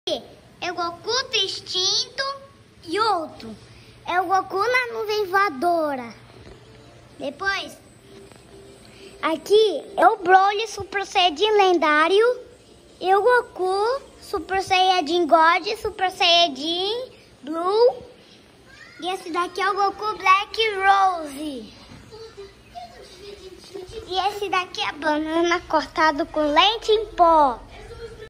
MAIS ALGUNS BONECOS DO DRAGON BALL QUE EU AMO / SOME DRAGON BALL TOYS THAT I LOVE
Tenho dois funko pop e três action figures, curtam meu vídeo pra eu fazer unboxing. Obrigado!!!